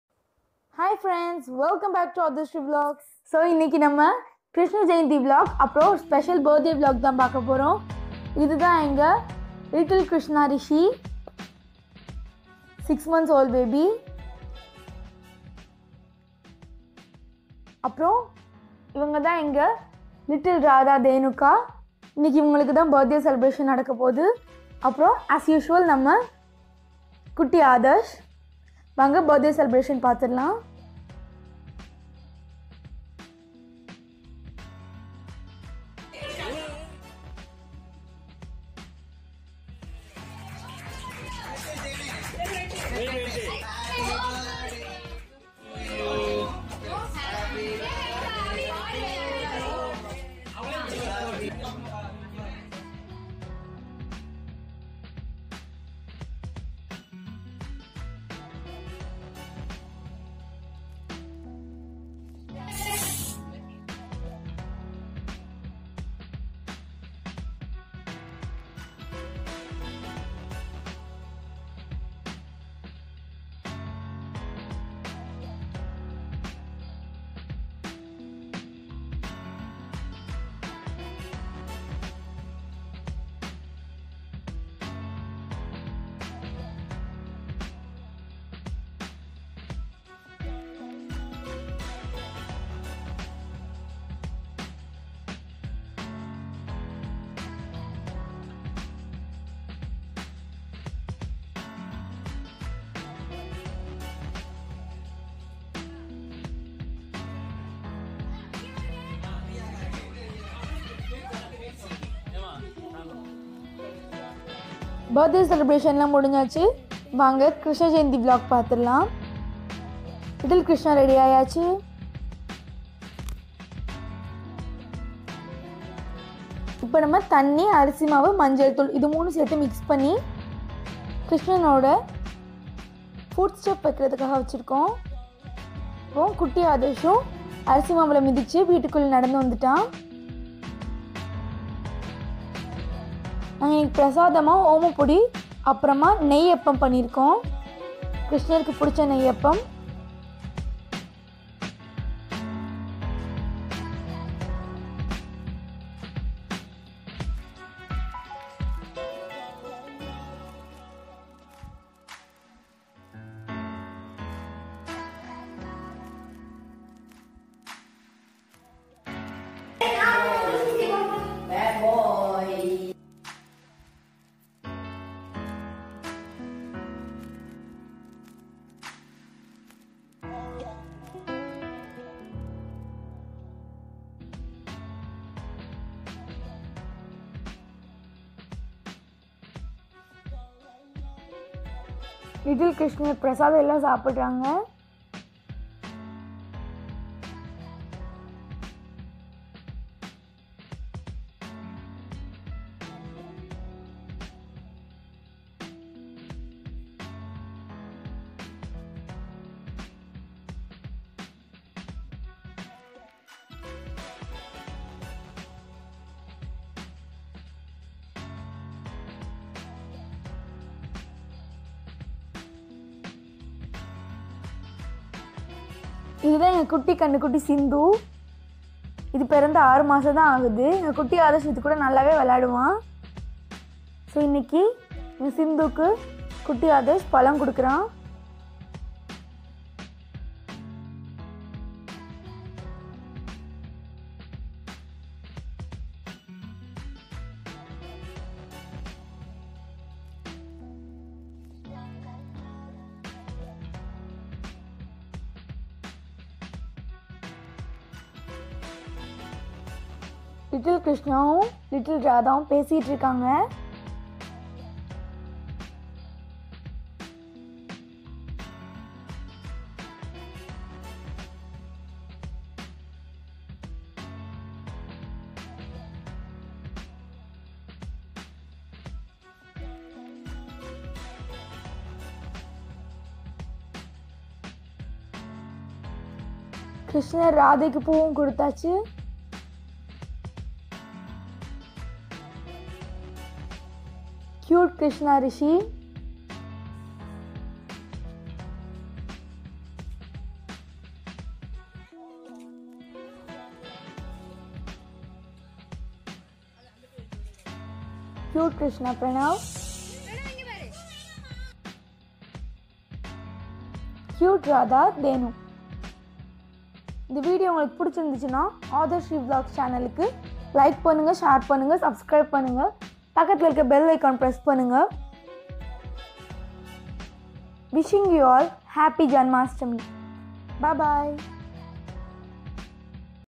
கிருஷ்ணா ஜெயந்தி || Krishna Jeyanthi Celebrations and kutti Radha (Dhenuka's) Birthday Celebs Vlog
Hai Friends !

                In this video we celebrates krishna jeyanthi and kutty radha (Dheuka's) birthday. Hope you all enjoy this video . Keep supporting us!!! Stay home!!!

Thankyou!